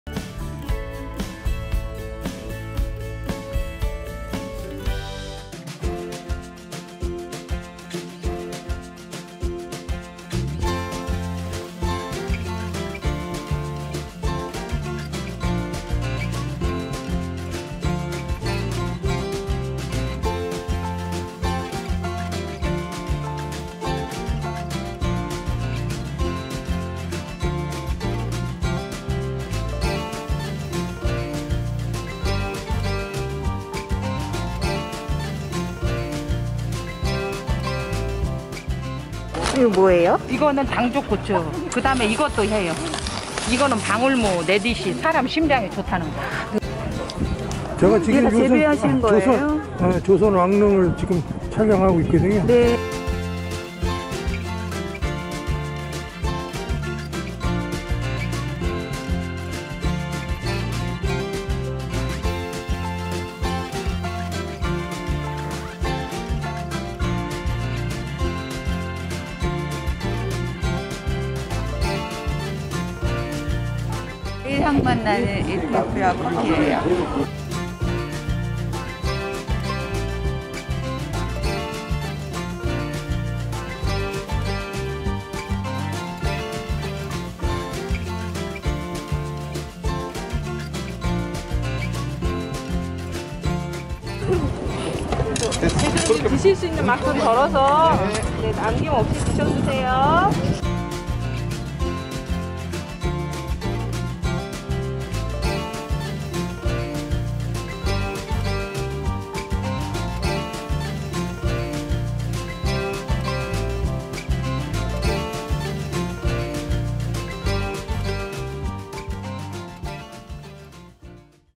[aT SNS서포터즈] 농부와 대화하고 자연을 생각하는 시장! 두물:뭍 농부시장 방문했어요!
😀 SNS서포터즈​ 가 간다!!!😀

농부와 대화하고,
자연을 생각하는 시장
두물뭍농부시장 방문했어요!

🌽자전거길 북한강 철교 쉼터 아래
자리한 두물:뭍 농부시장!!

🌽 친환경 방식으로 농사지은 농산물이 가득!
당일 재배한 농산물에서 신선함이 느껴져요!

🌽생산자와 직접 이야기를 나눌 수 있는데요.
친환경 농사를 짓는 농부의 자부심을 느낄 수 있답니다.

🌽직접 재배한 농산물로 만든 먹거리도 만나볼 수 있어요!
전통시장과는 또 다른 매력의 농부시장!
아이디어 식품들이 가득가득해요!

개인 식기도구를 가져오지 않았을 경우 대여도 가능하답니다!

🎲주소 : 경기도 양평군 양서면 양수리 521번지
🎲손 소독, 마스크 착용은 필수!!